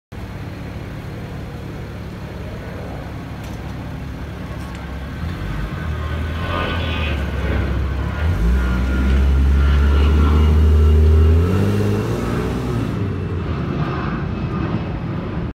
Signature Custom wraps & Coatings BMW Z4 project
BMW Z4. Matte white. Custom strips. Custom body kit installed with exhaust.